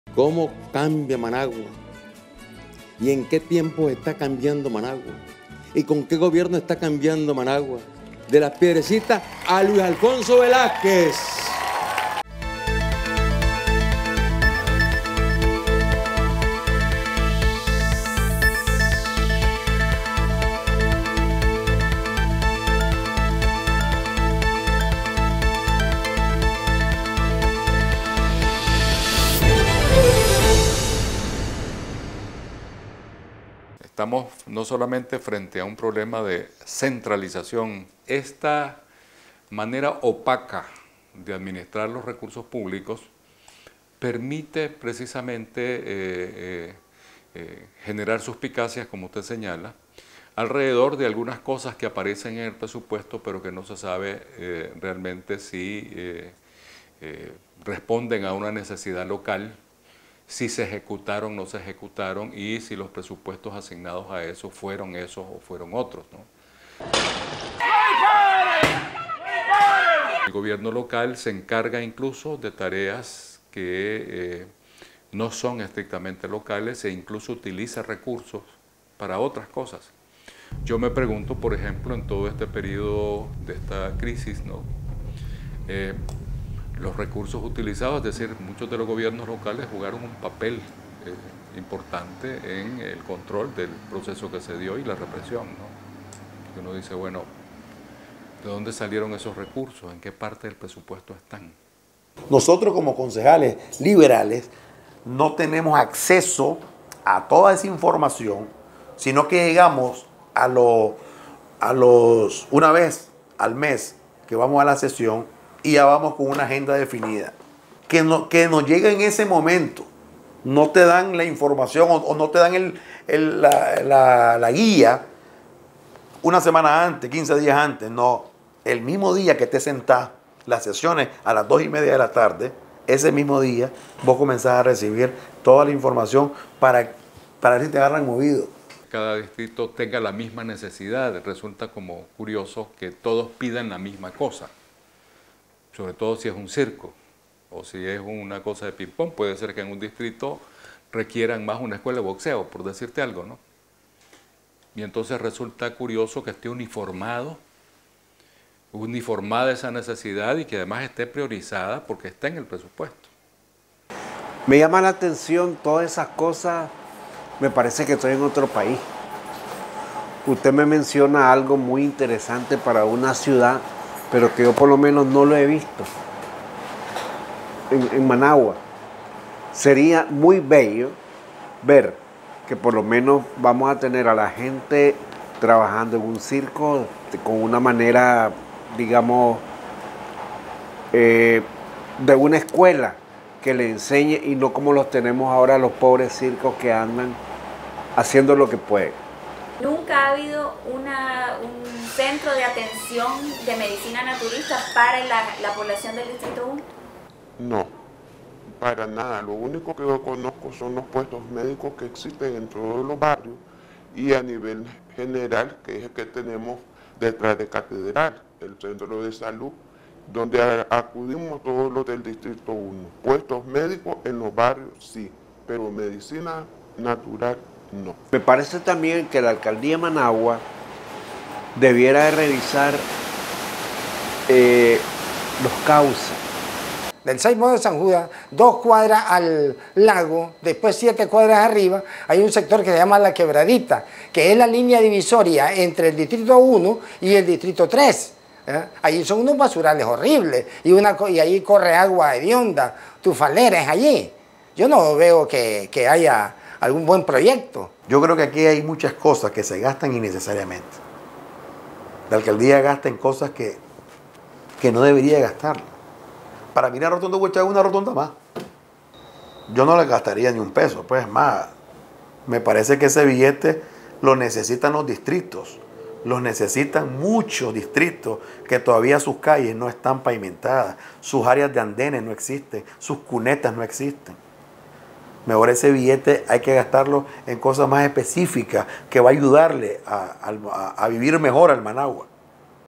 Los claroscuros del presupuesto municipal
Una investigación sobre las prioridades presupuestarias de la Alcaldía de Managua, la falta de transparencia y el bloqueo ciudadano a su derecho a participar en la gestión edilicia.